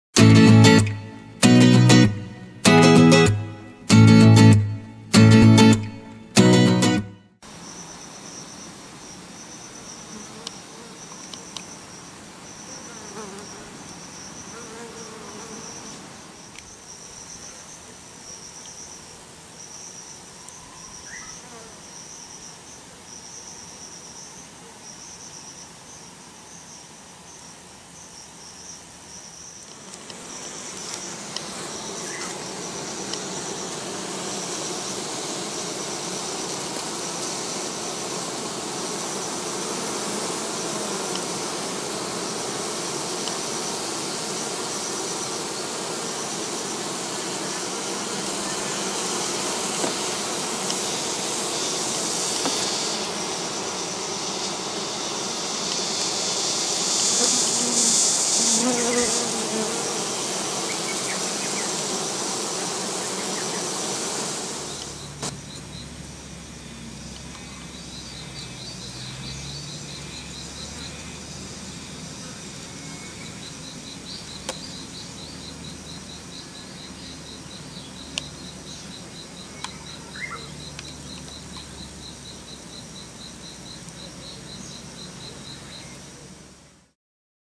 日本蜜蜂 100円ショップで購入した日除けで日本蜜蜂の巣箱を暑さ対策。 オーニングは使えるか検証
100円ショップに売っている株式会社武田コーポレーション社製ウインドオーニングを使用して日本蜜蜂の巣箱の日除け対策と暑さ対策ができるか検証してみました。
遮光率は79%の記載があります。
果たして結果は如何に！
ウインドオーニング：